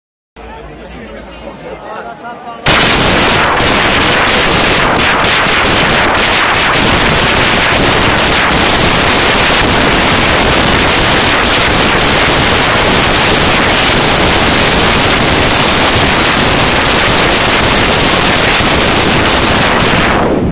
FERIA 2014. Tembleque. Traca de Fin de Fiestas.
A las 03:30am del día 28 de agosto, en la Plaza Mayor. Vídeo por cortesía de Sergio Fernández.